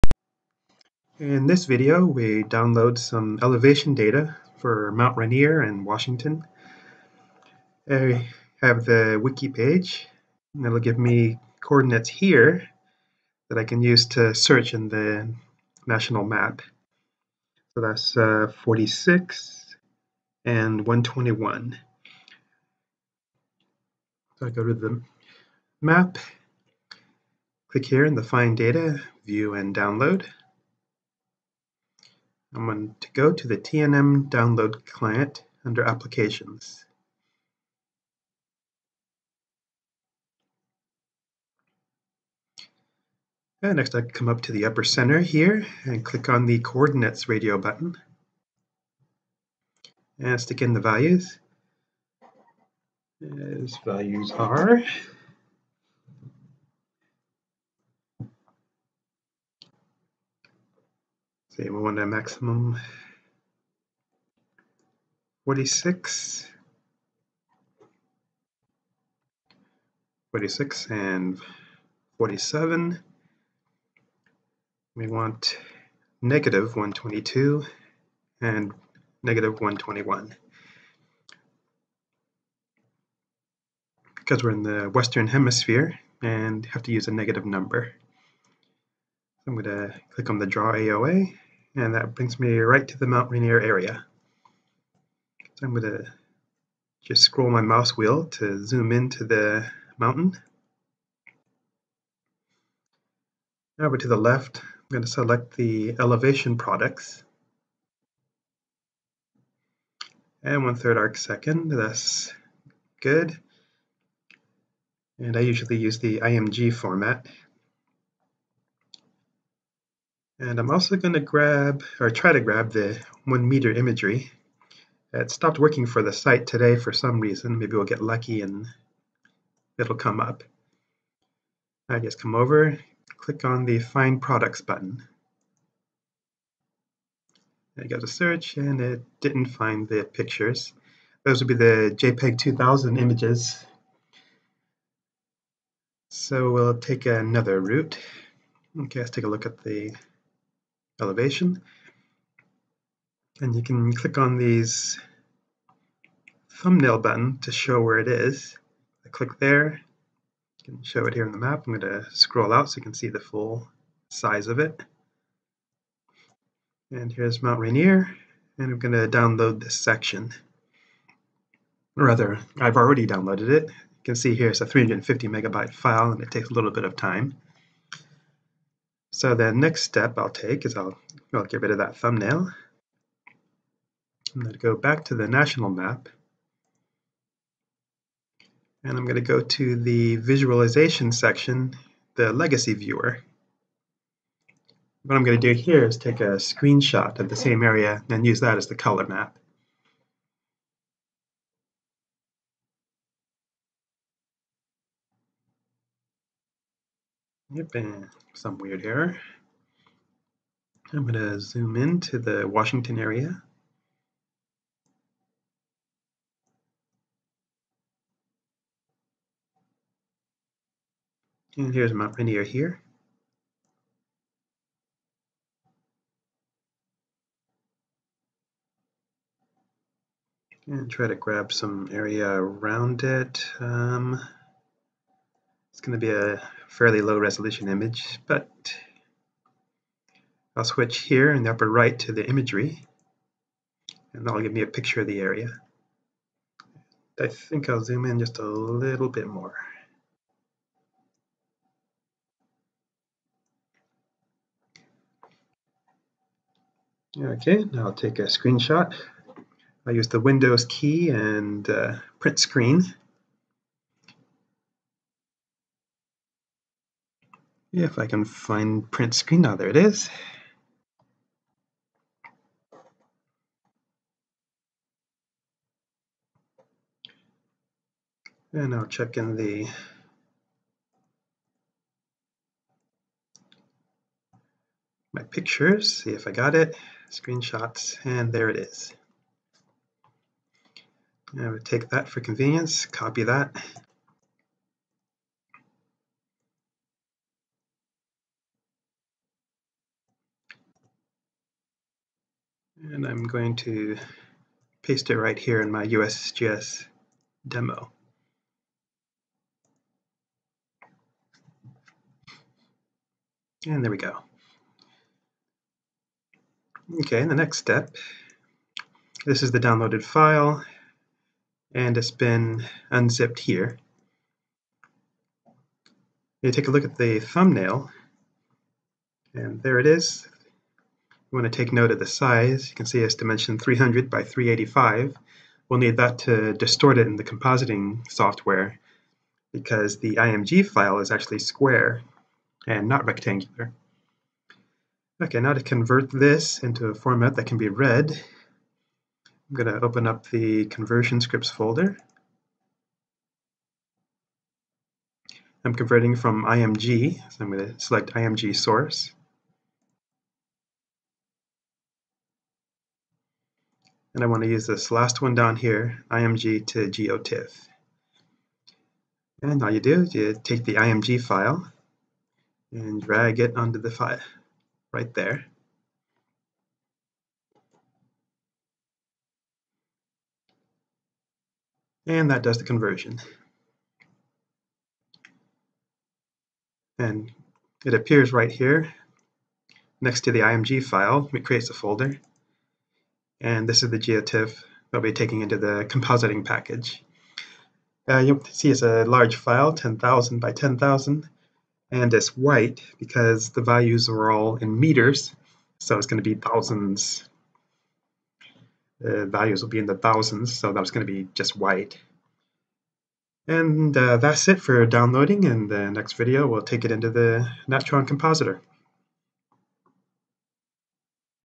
02 - map download
video series demonstrating a method for importing US Geologic Survey map data into the Unreal Game Engine using the Natron compositing software.

presented by Clinton Reese